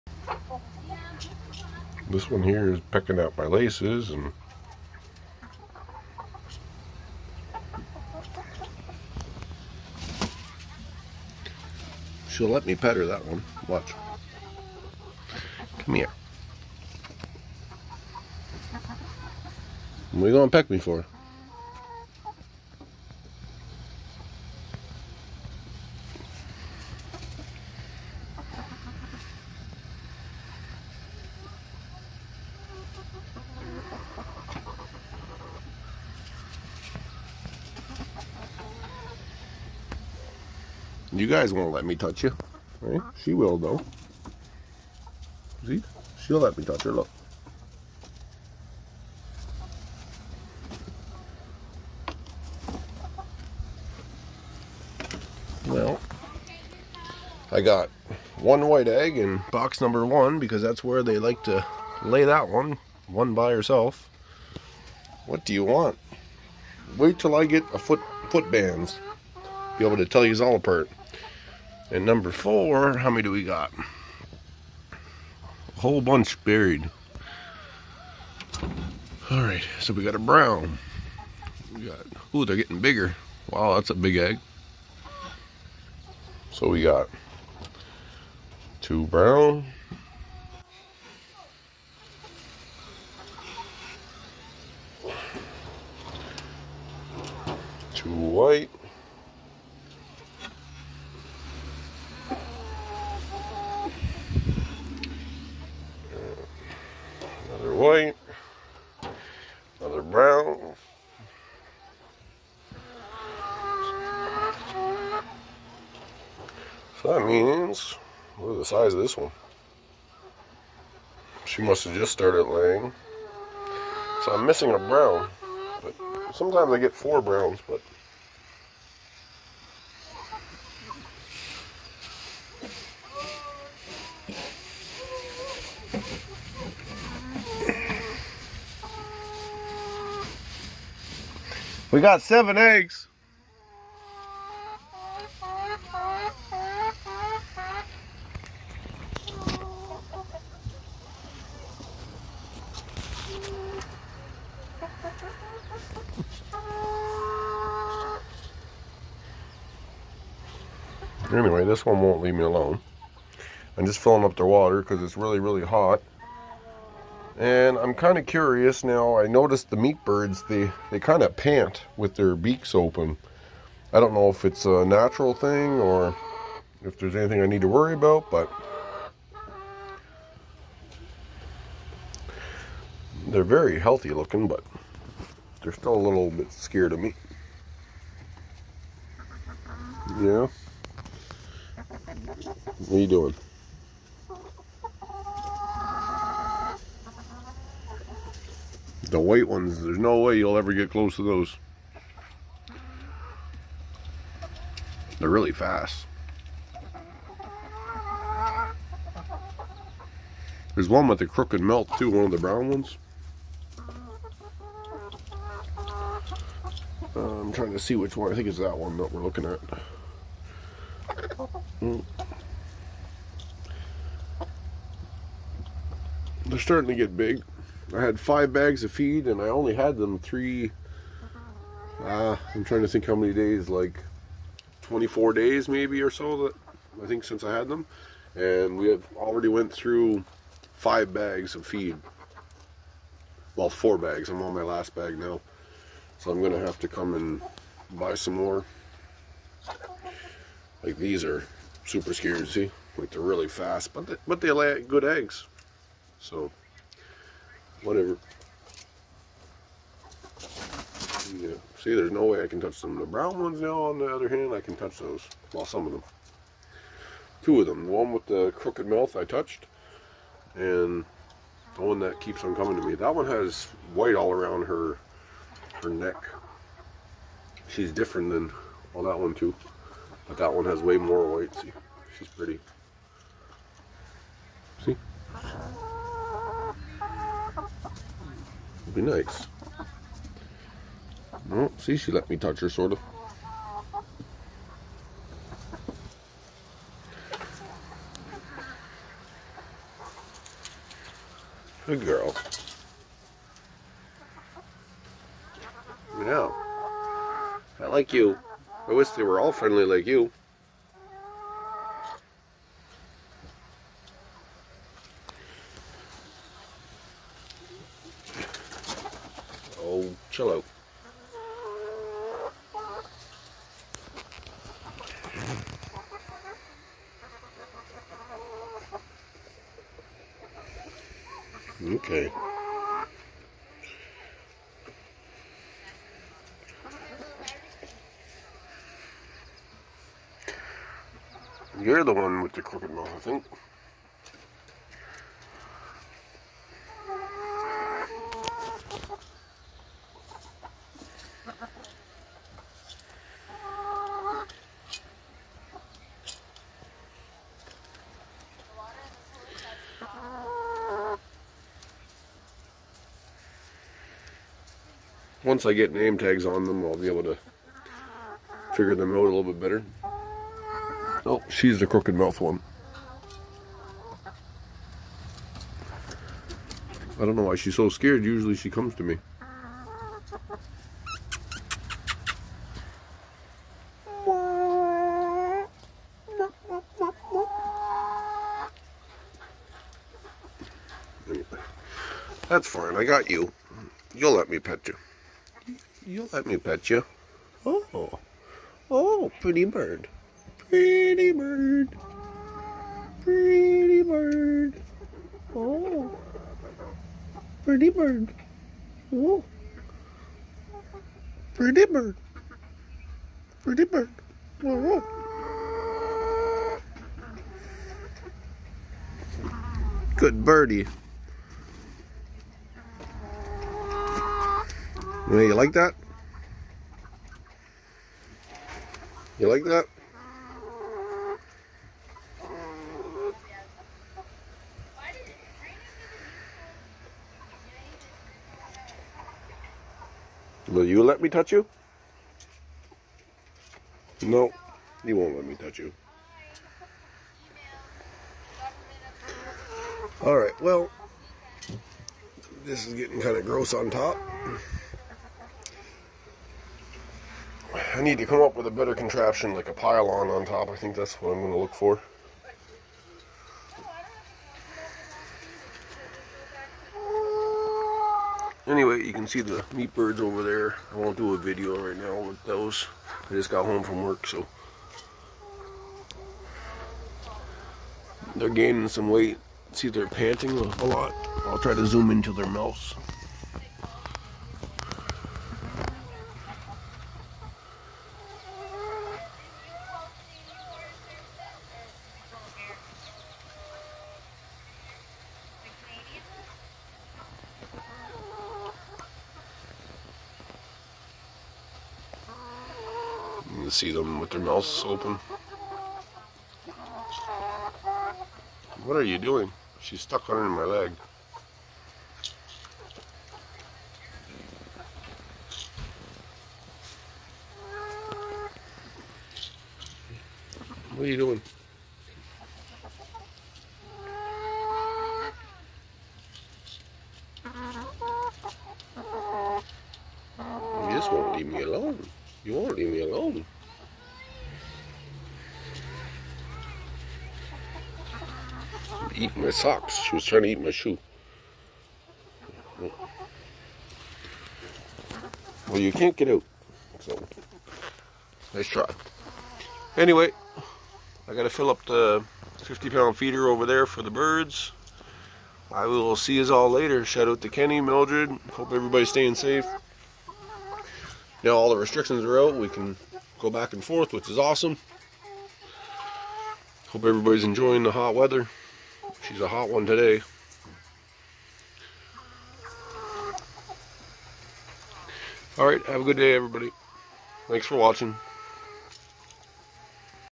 I finally got a new friend.
loving the brown hens. so much more friendly then the white ones.